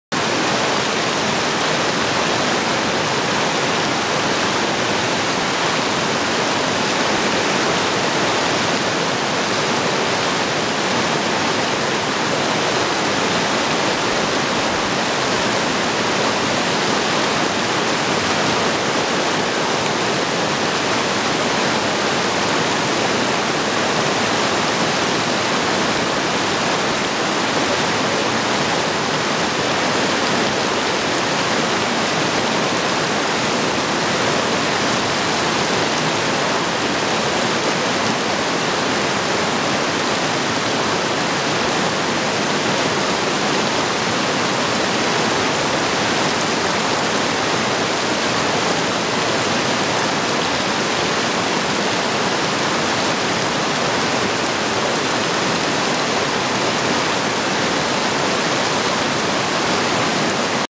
buckeye hotsprings
buckeye hotsprings near bridgeport, ca on a cold morning.